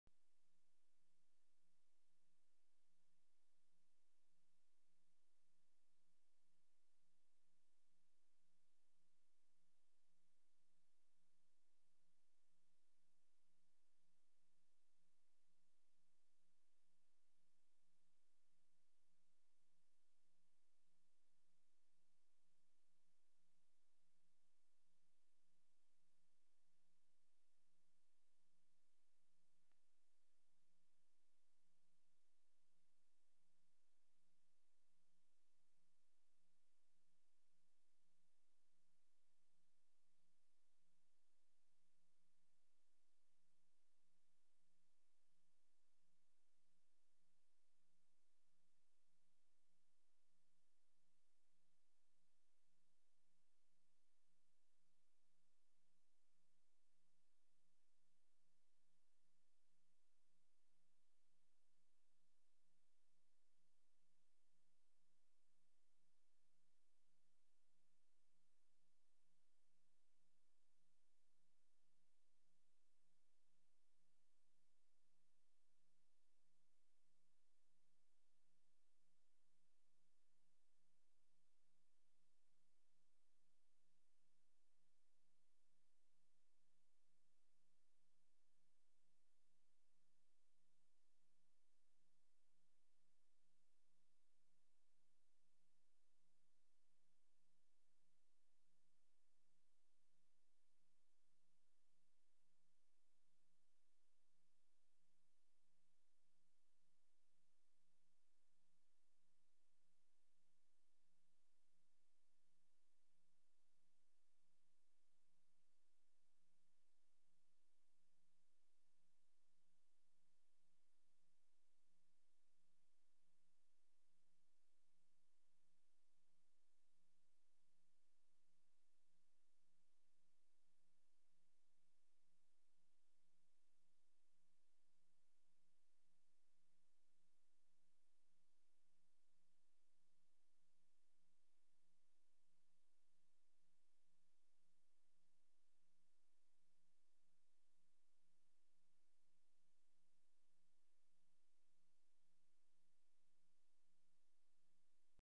FRENCH STRIKES - NO SOUND
Various shots of troops occupying the pits. Troops marching into pits. Some mobilised, on half-track trucks. Troops march past camera.